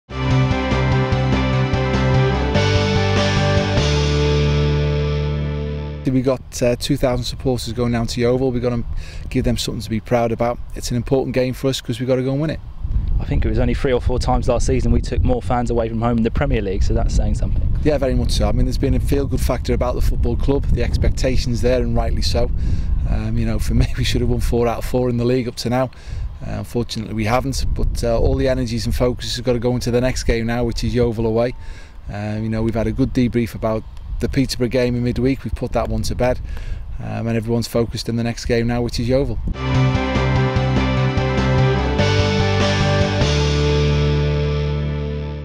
Adkins on the travelling thousands | Yeovil vs Reading | 31.08.13
Reading FC boss Nigel Adkins delighted with the travelling horde of over 2,000 Royals fired up for trip to Yeovil in the Sky Bet Championship - full interview can be found